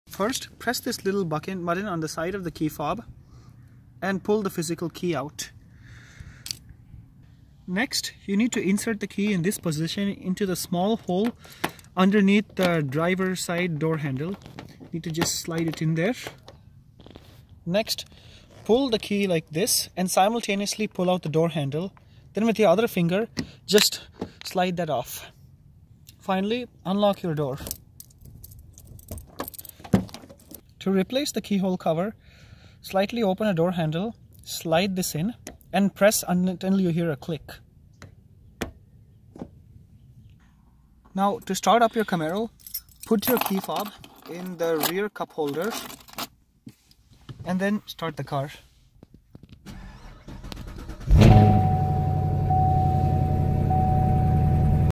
How to Unlock and Start Up Chevrolet Camaro (6th Generation) with Dead Key Fob Battery
Key fob for your Camaro dead? Watch this quick video to learn how to unlock and start up your Camaro!